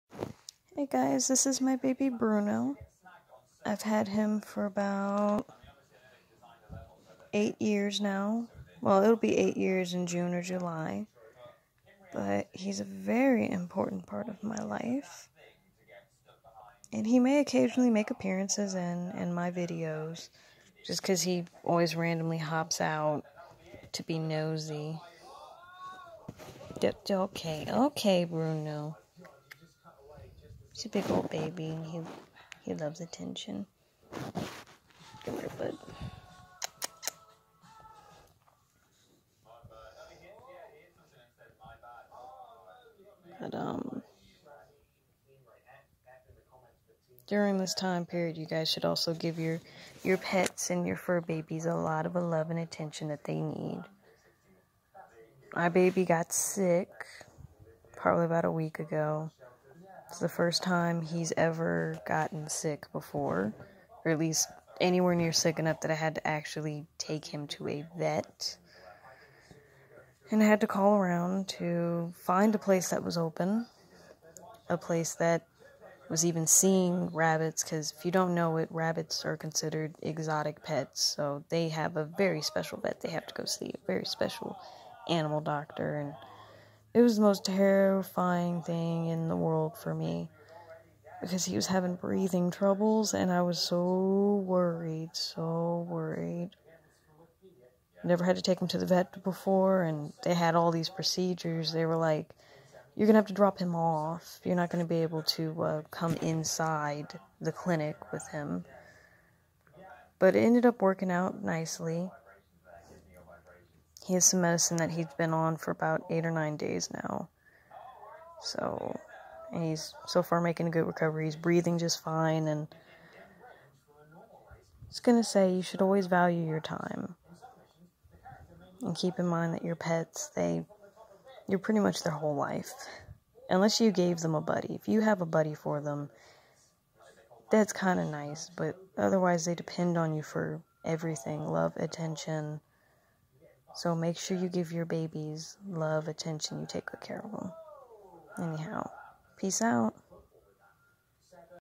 My Furbaby Bruno
Here I'm just introducing my baby Bruno, he is a 8 year old Flemish Giant Rabbit and my child for intent and purposes. He may sometimes make an appearance in my videos.